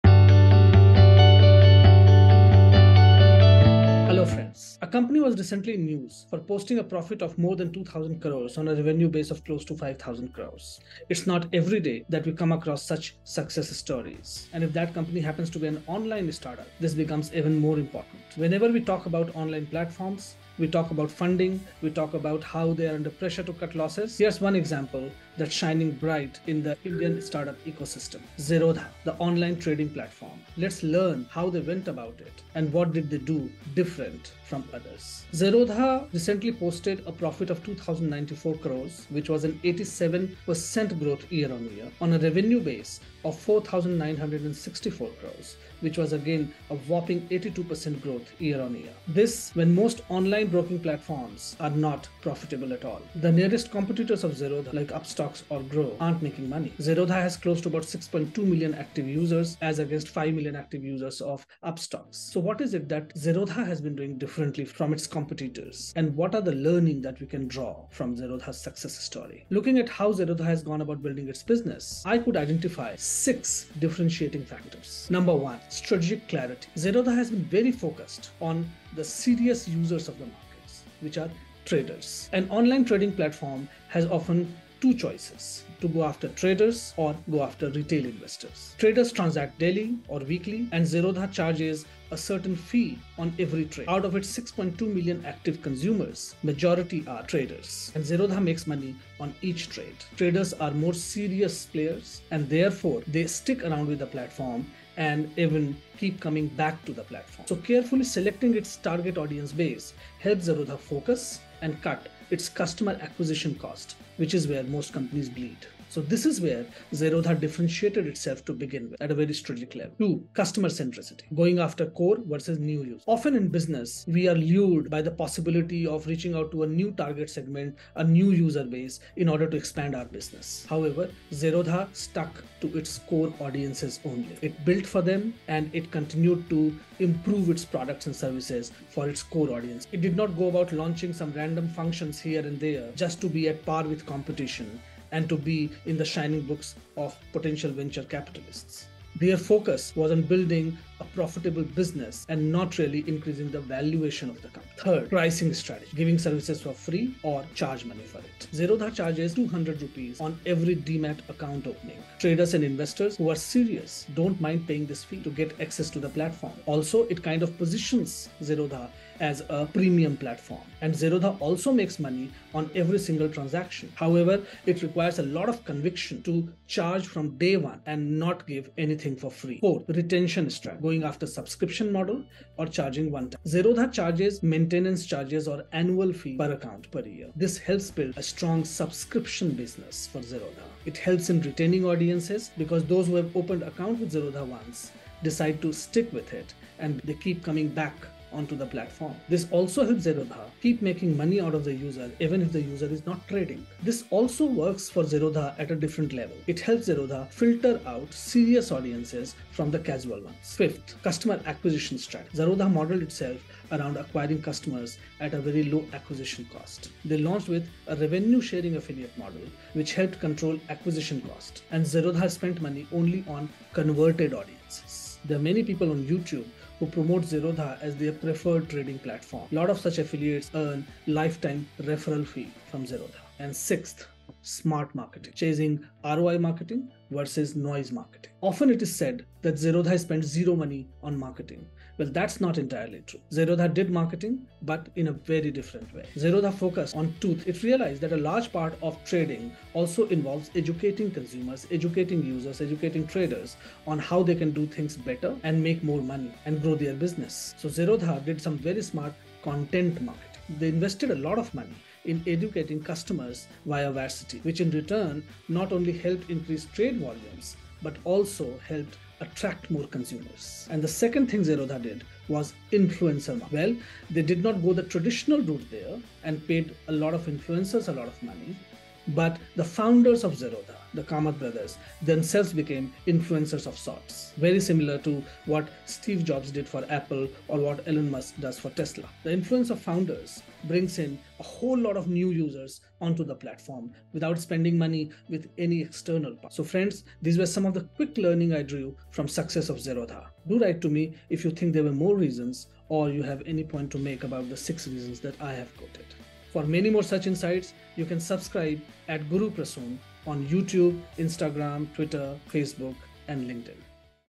How Zerodha built a Rs. 2000 crore Profit business : 6 Lessons
Online Startups are generally associated with skyrocketing valuations, VC fundings and more recently their high cash burning business model. In this backdrop, one Indian fintech startup, Zerodha, stands out as an outlier and is a role model for many budding entrepreneurs & startup enthusiasts. So, how did Zerodha buck the trend? What did they think & do differently to be so successful? Here I decode 6 lessons from their journey. Do write to me if you have some more and keep enriching the discussion.